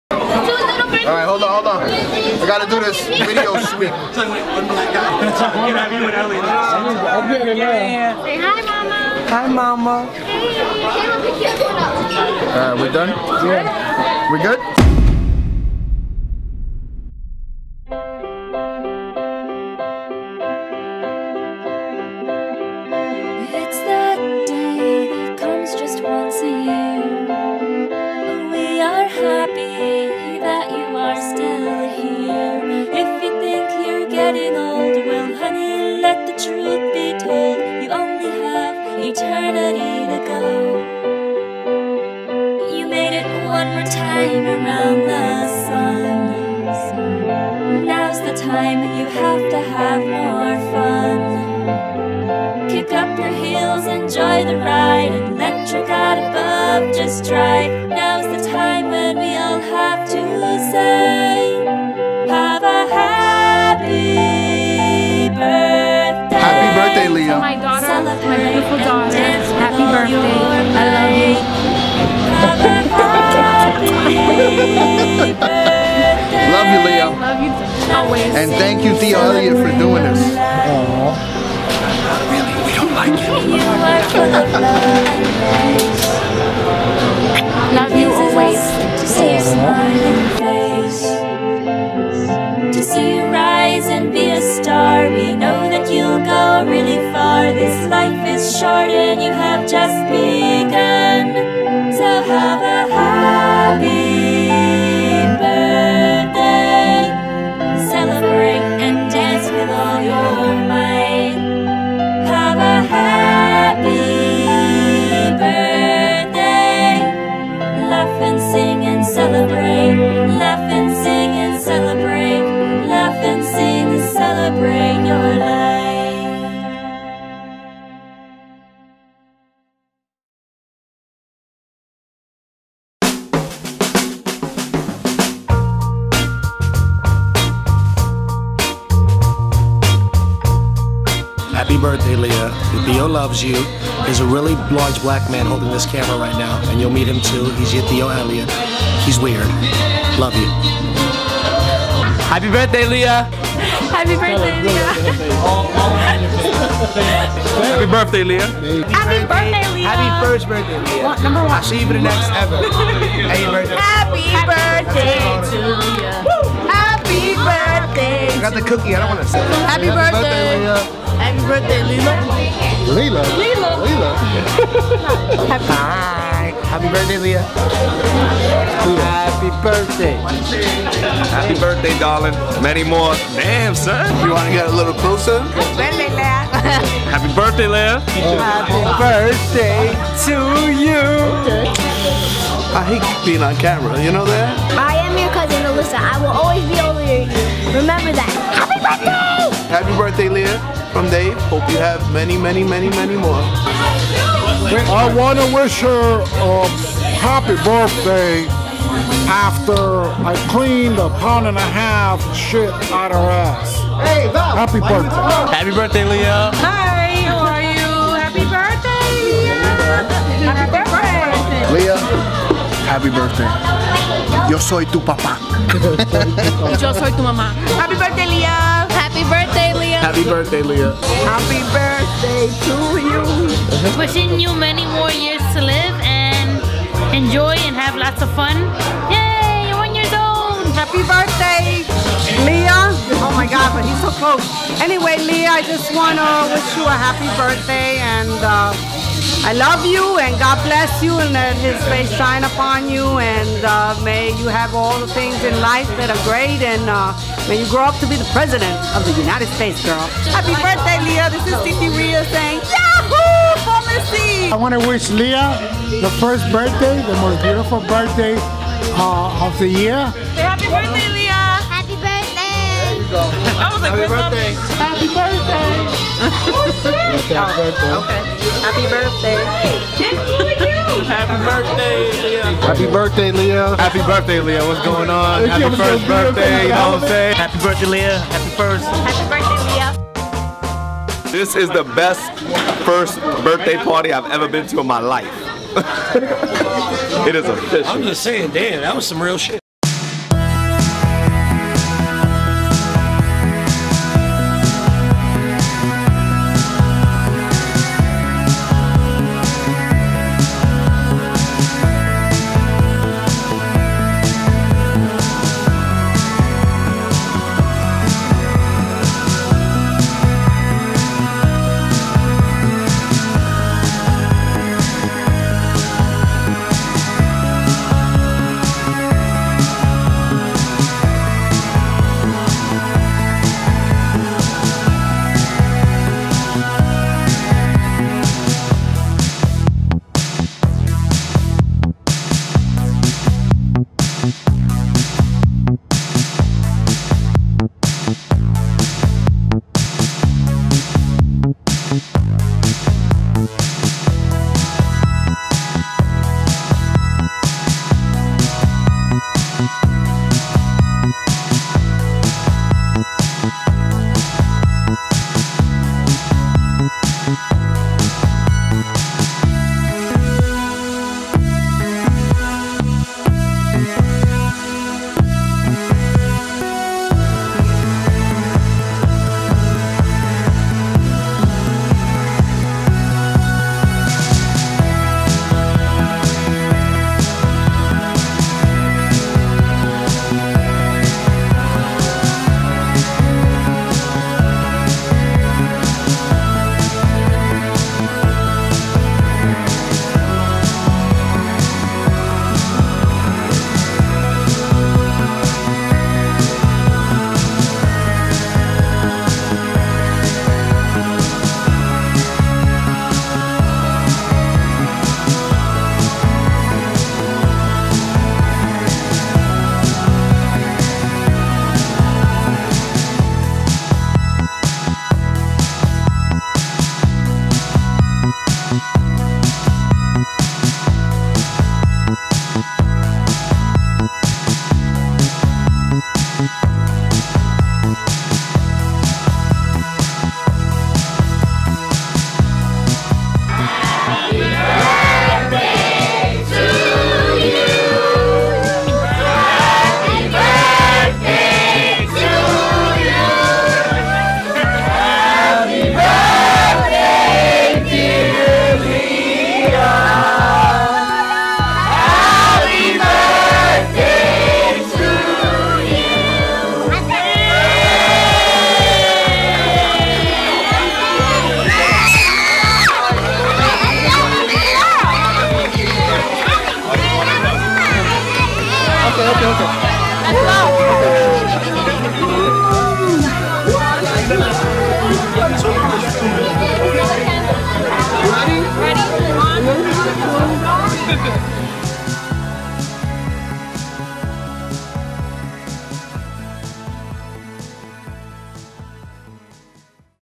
LeahBday1
Video I did for a friends Daughters first birthday.

Shot using Canon 650D with kit 18 - 135mm lens

Tools used : Kessler DollyV2.0, Kessler Cranke KC 8, GlideCam HD2000